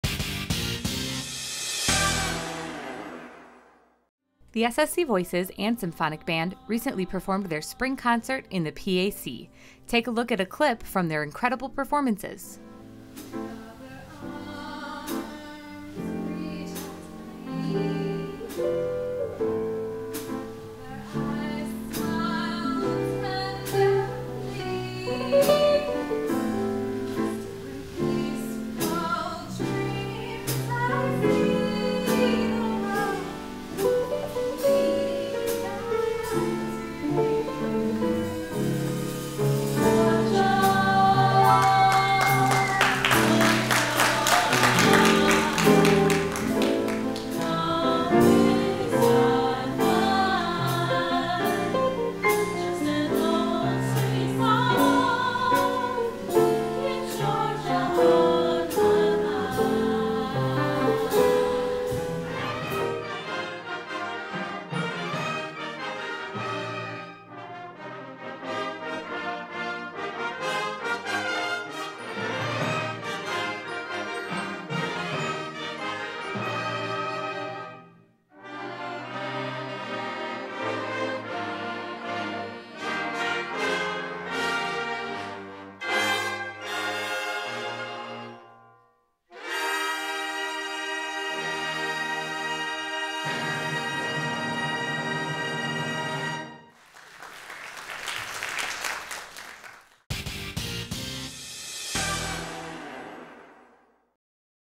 SSC Voices & Symphonic Band Concert - Bulldog Bites
Take a look at some clips from the SSC Voices and Symphonic Band Concert.